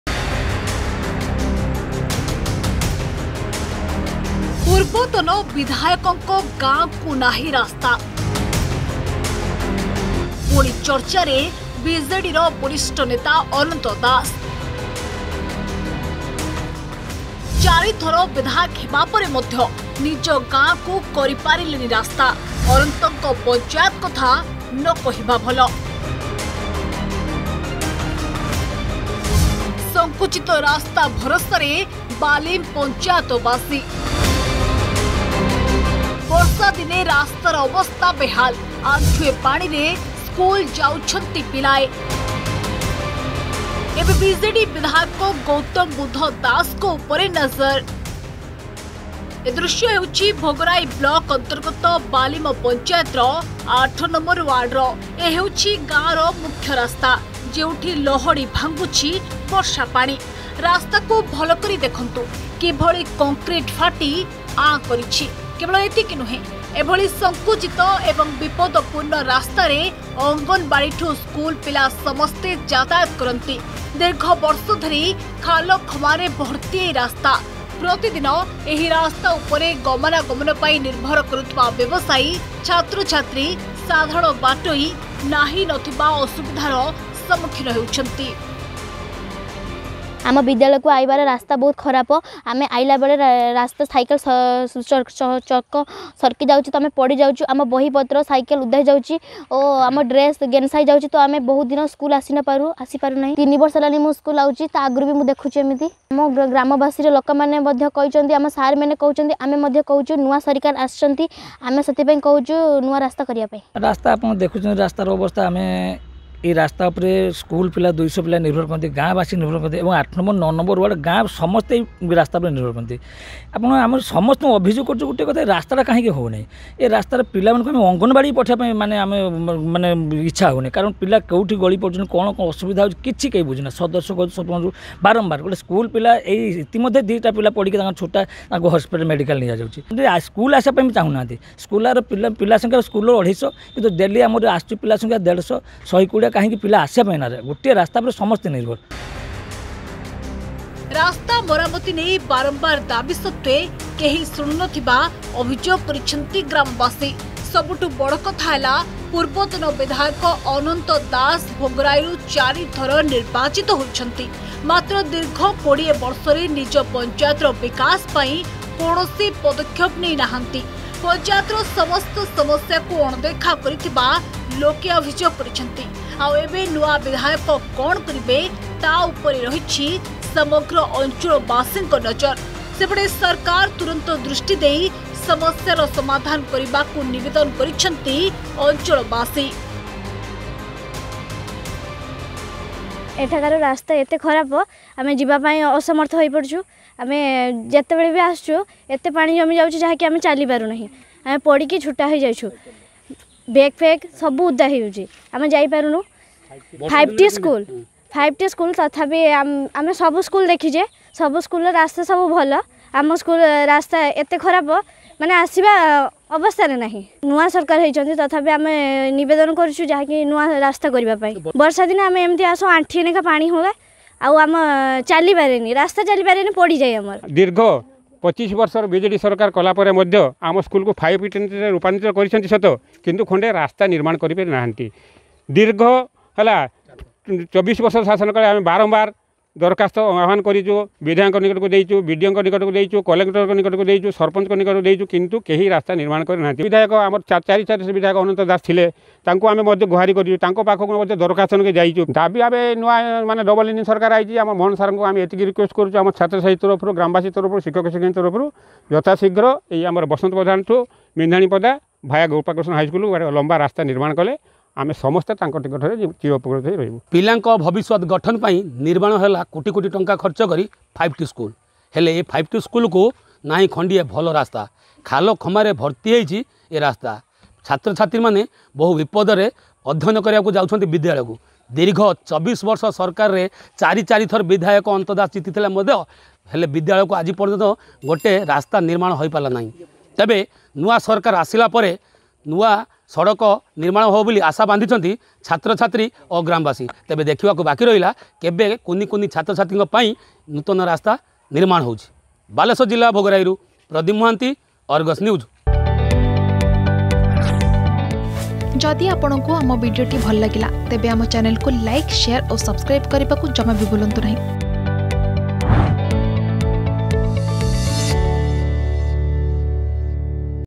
ରାସ୍ତା ପାଲଟିଛି ମରଣା ଯନ୍ତା || Road problem || Bhagarai
ରାଜ୍ୟରୁ ବିଜେଡି ସରକାର ହଟିଛି କିନ୍ତୁ ଲୋକଙ୍କ ଦୁଃଖ ହଟି ନାହିଁ। ଆଜି ବି ଗାଁକୁ ପାଦ ଚଲାରାସ୍ତା ନାହିଁ ଖାଲଖମା, ପାଣିରେ ଭର୍ତ୍ତି ରାସ୍ତା। ଏ ହେଉଛି ଭୋଗରାଇ ବ୍ଳକ ଅନ୍ତର୍ଗତ ବାଲିମ ପଞ୍ଚାୟତ ୮ନମ୍ବର ୱାର୍ଡ ର।